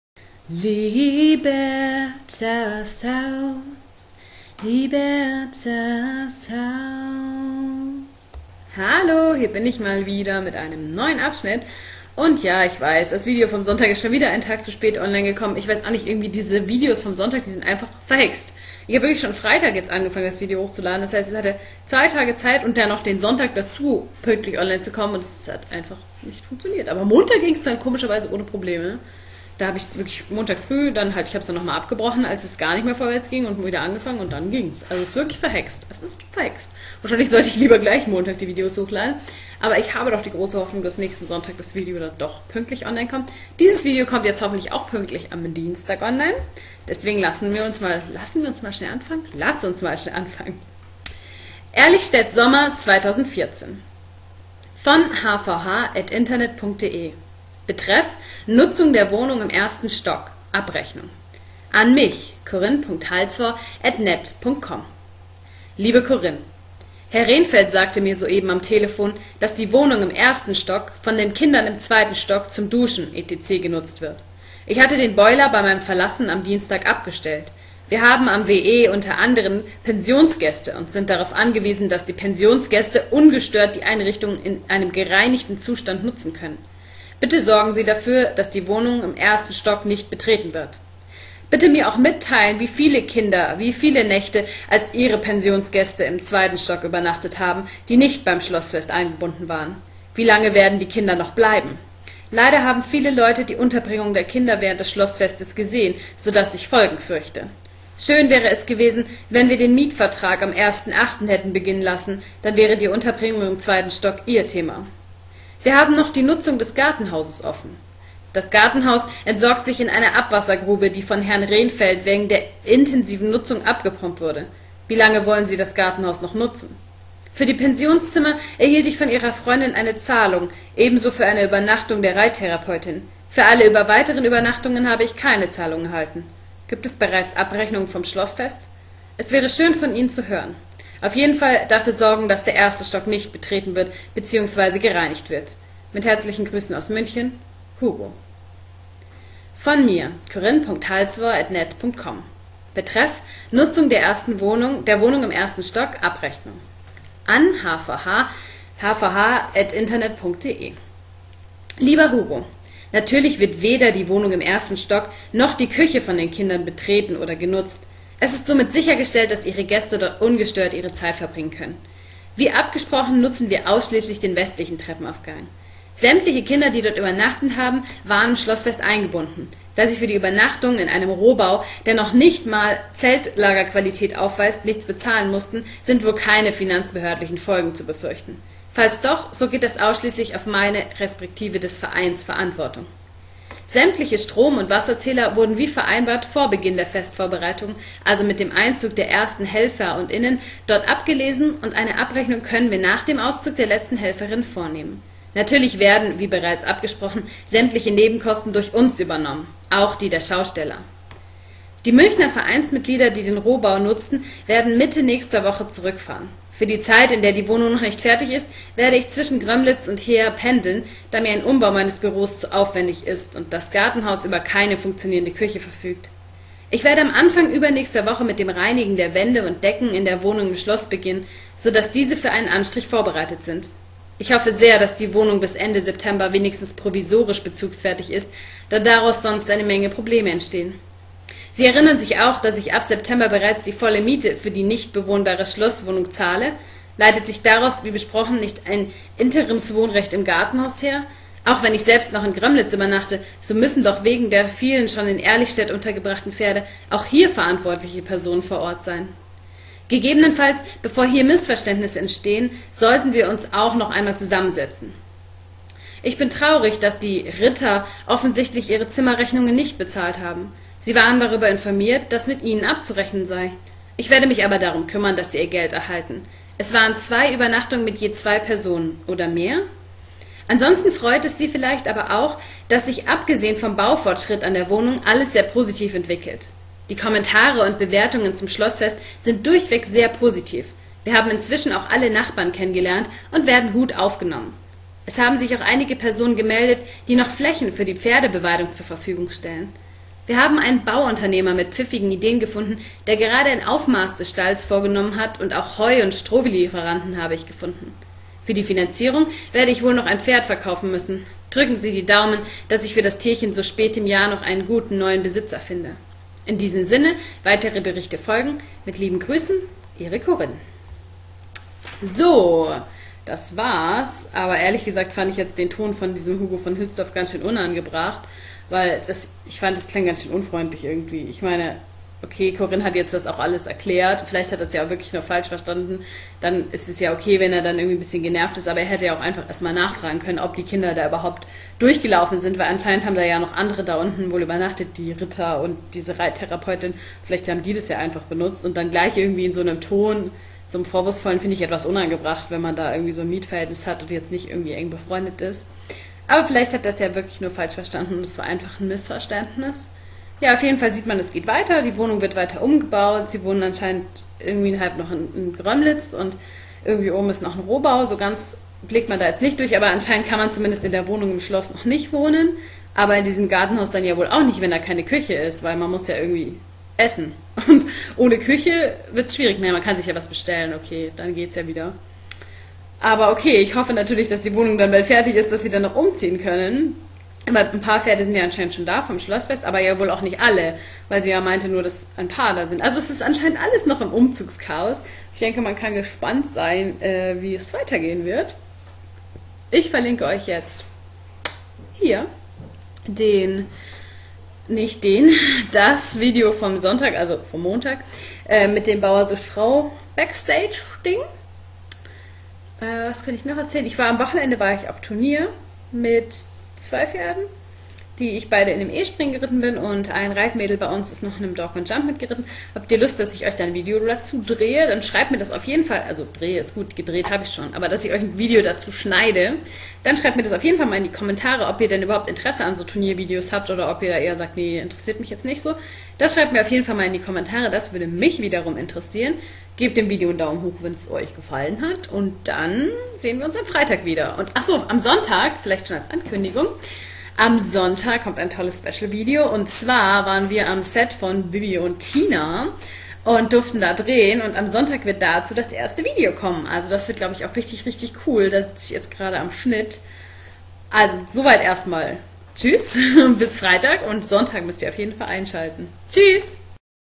144. Abschnitt ~ "Abrechnungen"
Bisher:
Corinne hat sich entschlossen, das Angebot von Hugo anzunehmen und in sein Schloss zu ziehen. Allerdings erläuft der Umzug und das Schlossfest ganz schön chaotisch.